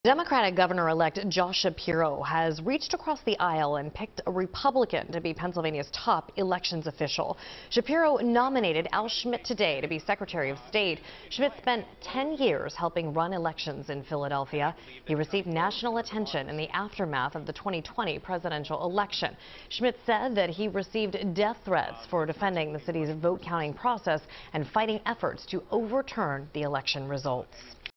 Shapiro nominates Al Schmidt as Pennsylvania's secretary of state
Schmidt, a Republican, spent 10 years helping run elections in Philadelphia.